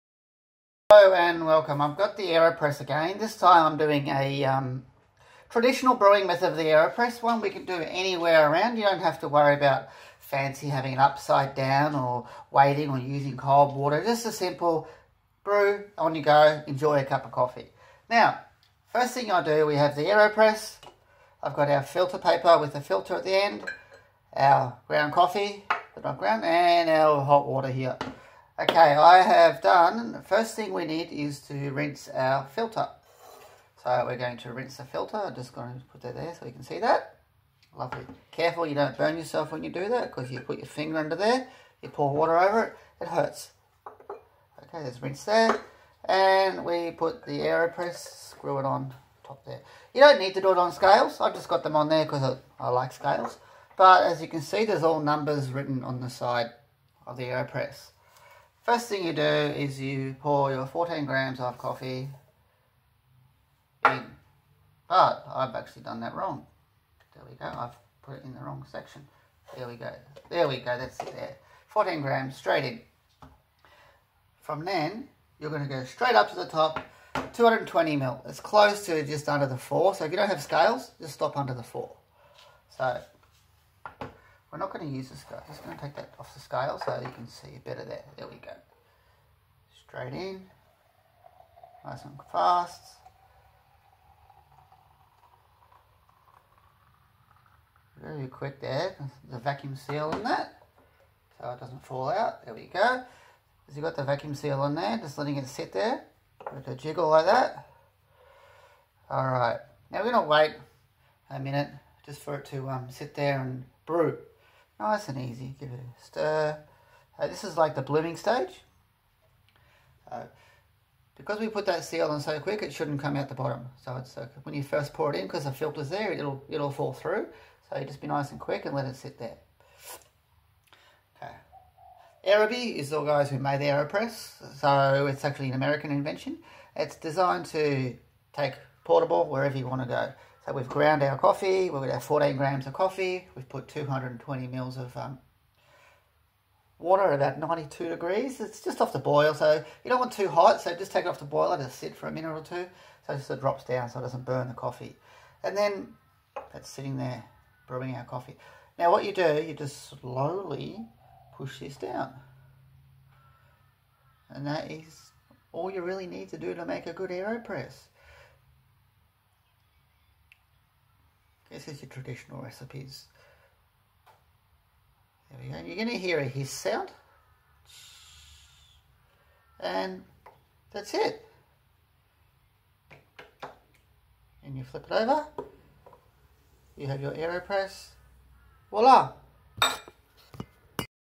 Traditional AeroPress method
Brewing the AeroPress the traditional way.
Need to have :
16gm coffee slightly corse ground
220ml water
Wash filter to remove the chemicals in the paper,
Add coffee to Aeropress
Top up quickly to just under the 4mark on the Aeropress (should be 220ml) then put the tube in place to create a vacuum. This will stop the water from coming out of the filter.

Wait for a minute for the coffee to bloom
Then press directly onto the cup.